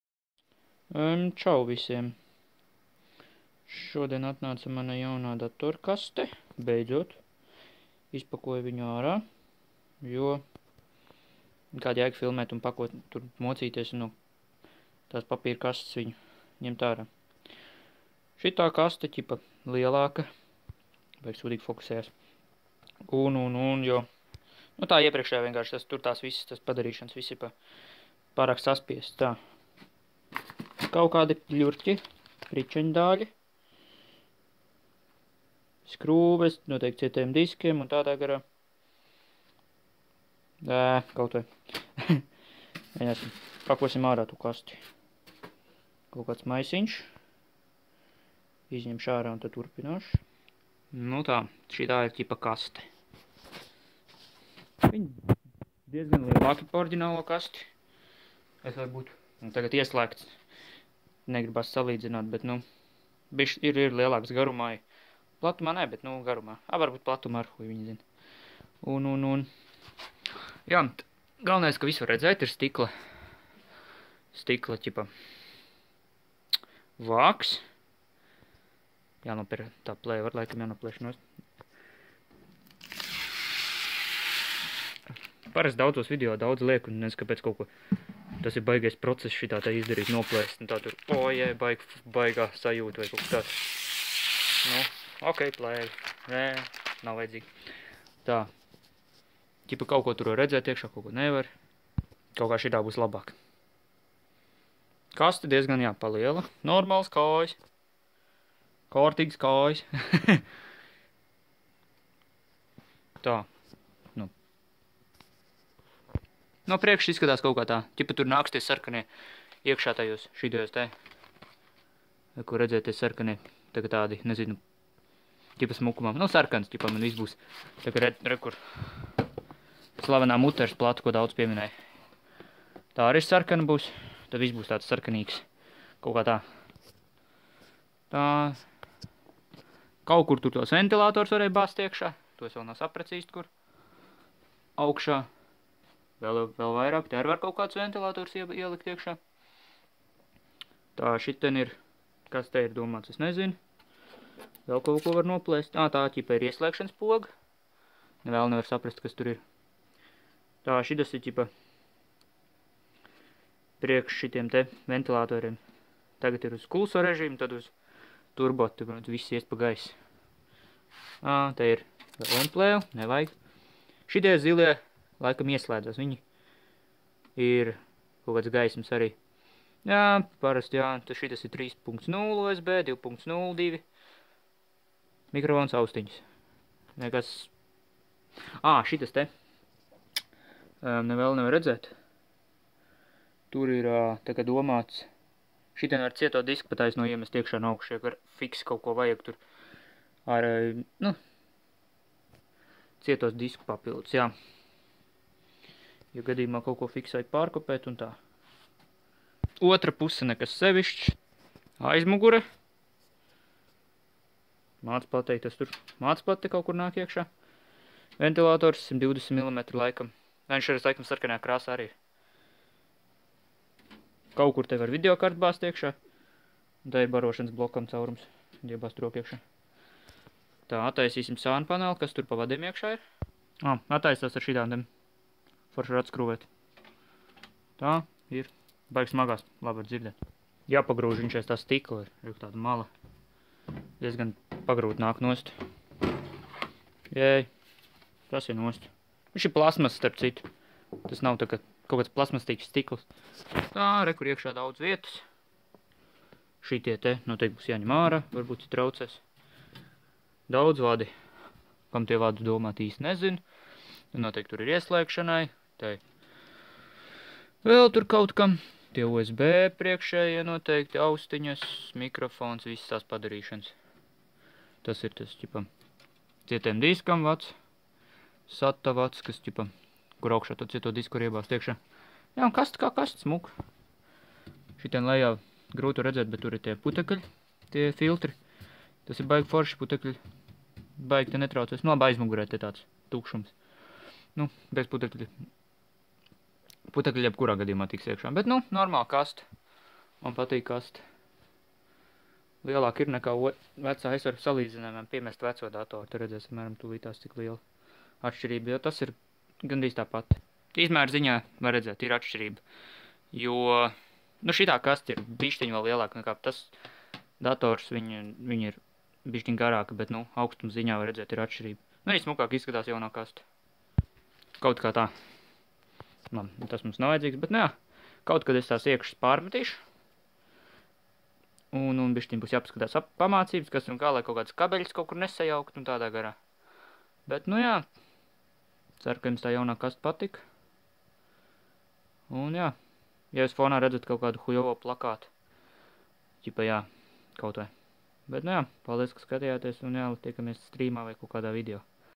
Jaunas dator kastes unboxings :D
beidzot pec 3 nedelu gaidisanas kaste atnaca. :D
atvainojos par video kvalitati,filmeju ar citu telefonu

te var nopirkt ja intrese